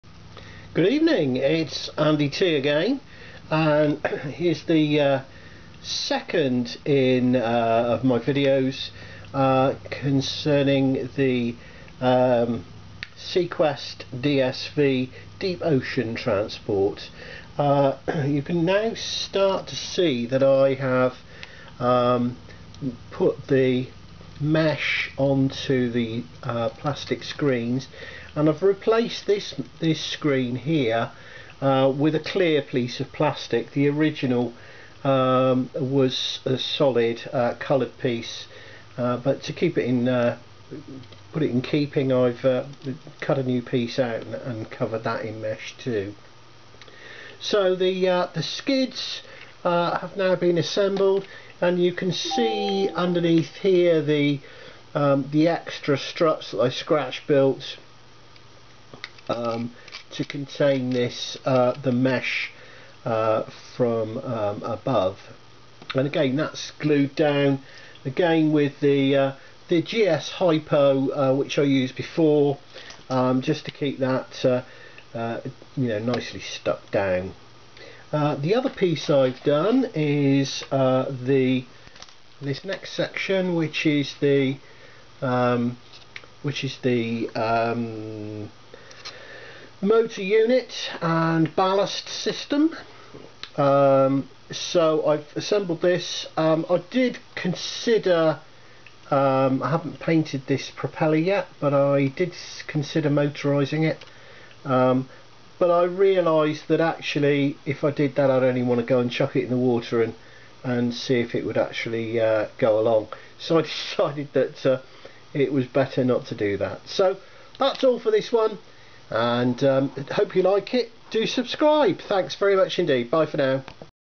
044 - 1:32 SeaQuest DSV Deep Ocean Transport - kit build part 2
Kit build log part 2 -  cargo bay completed and put on chassis